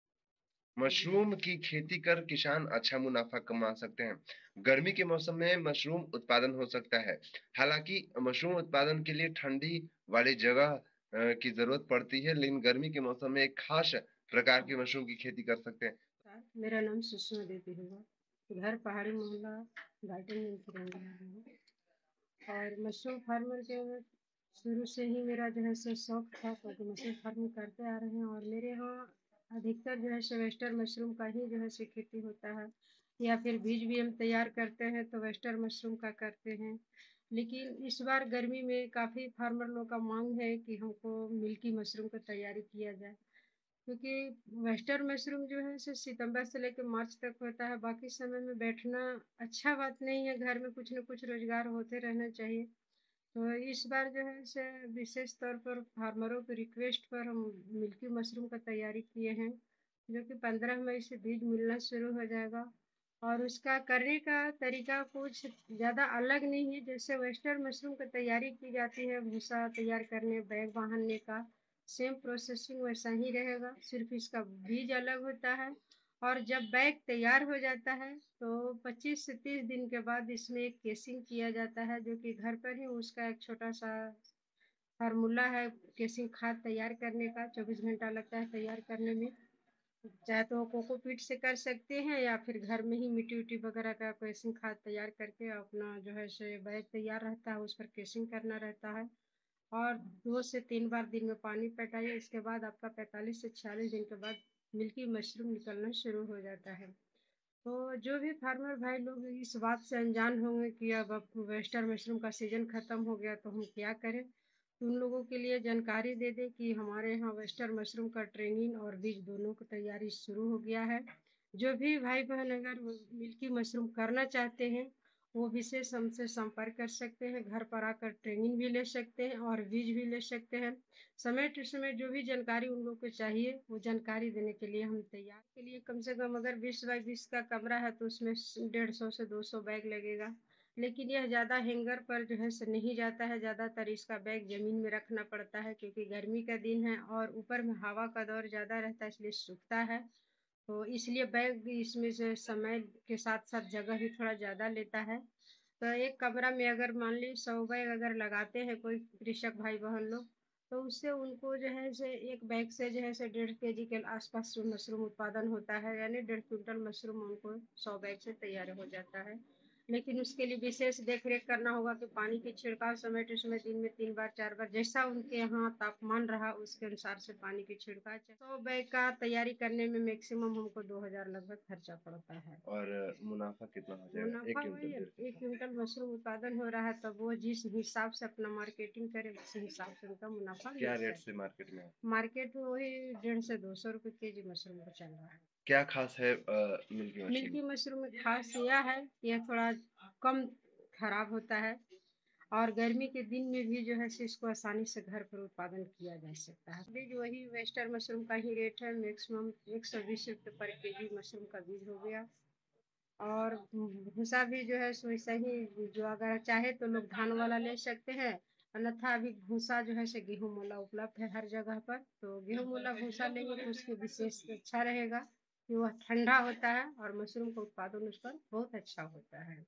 Mushroom Farming: इस तकनीक से मशरूम की खेती कर किसान कमा रहें लाखों!
Mushroom cultivation has made many people self-reliant. You also do mushroom cultivation. And if you are sitting idle during the summer season, then you can cultivate a special variety of mushroom, Milky Mushroom. You can get good benefits from its cultivation. Can earn profits.

मशरूम की खेती ने कई लोगों को आत्मनिर्भर बनाया है.मशरूम की खेती आप भी करते है.और अगर गर्मी के मौसम में खाली बैठे रहते है.तो आप मशरूम की एक खास प्रभेद मिल्की मशरूम की खेती कर सकते है.इसकी खेती से आप अच्छा मुनाफा कमा सकते है.